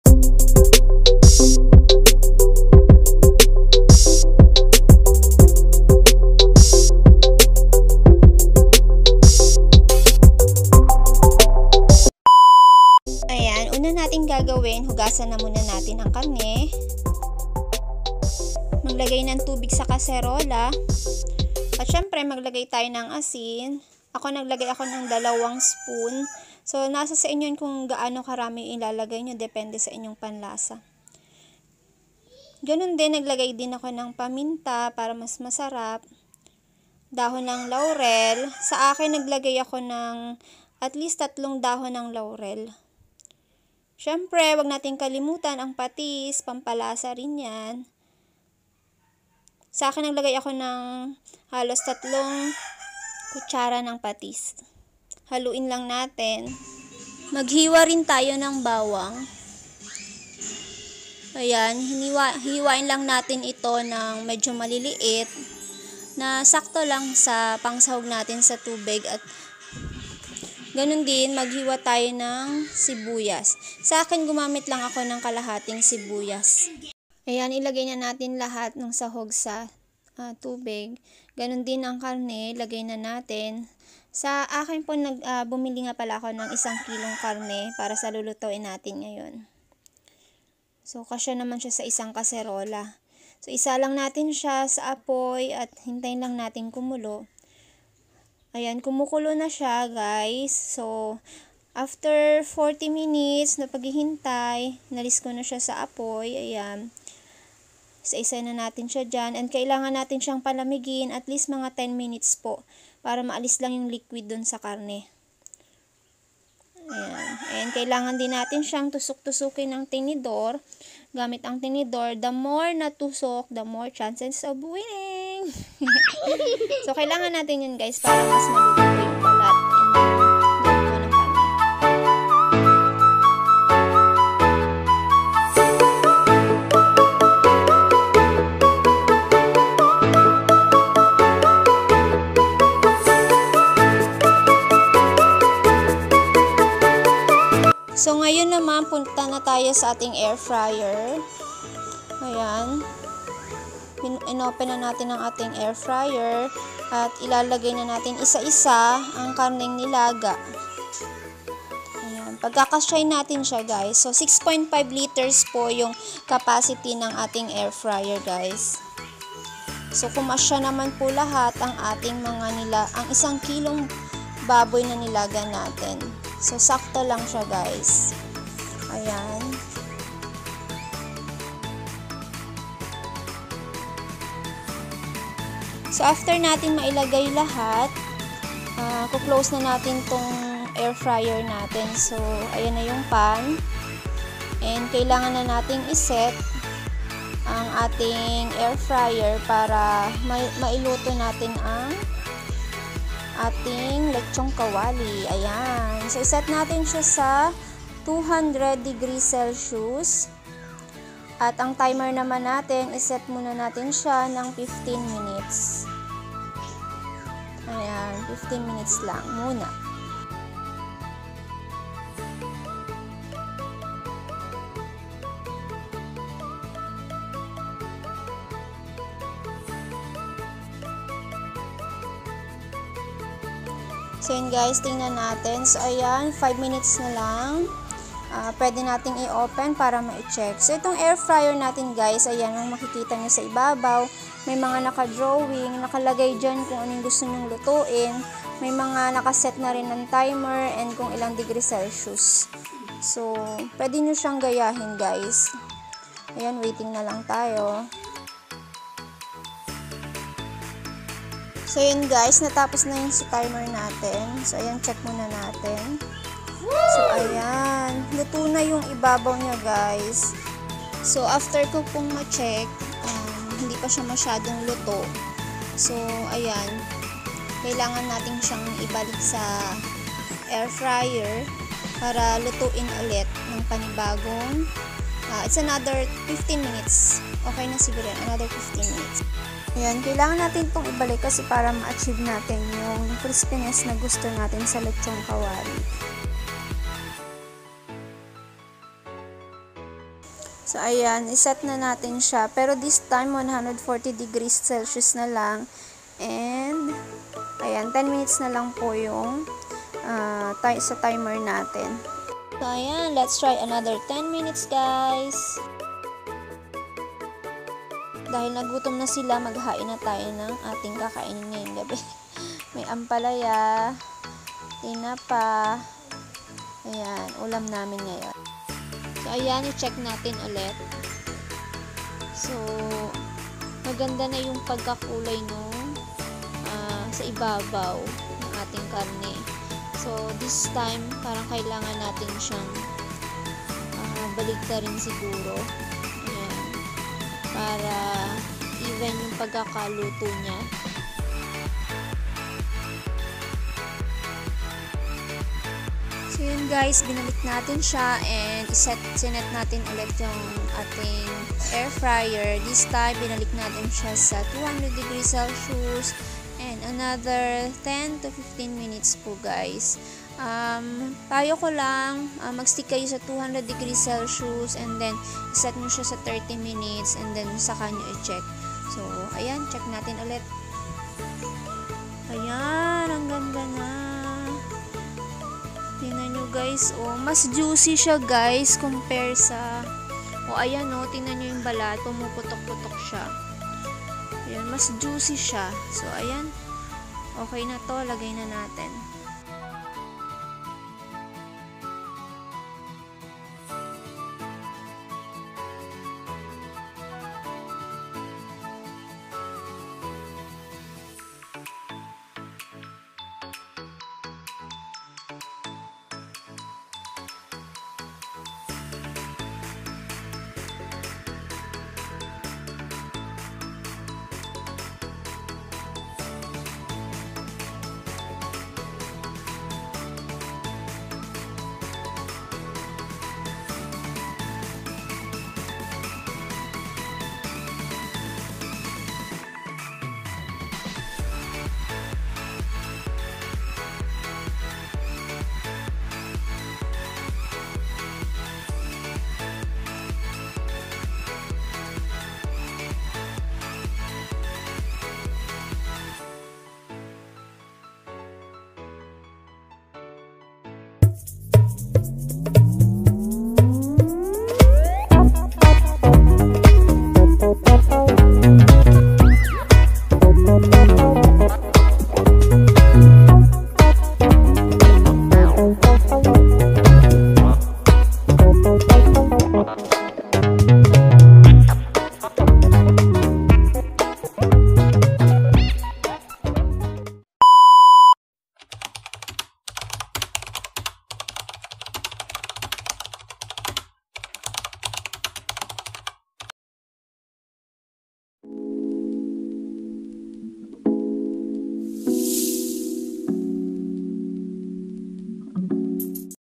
LECHON KAWALI RECIPE IN AIR FRYER | TESTING LANG | JANETGEECEEVLOG35
MAGLUTO NG LECHON KAWALI SA AIR FRYER!
Nagtry po ako magluto ng Lechon Kawali sa aking bagong biling Air Fryer,
Plus sinama na rin natin ang RECIPE ng LECHON KAWALI
at kung PAANO ito lutuin bago ilagay sa Air Fryer.

PANOORIN ANG PAG-UNBOX KO NG AIR FRYER NA NABILI NATIN SA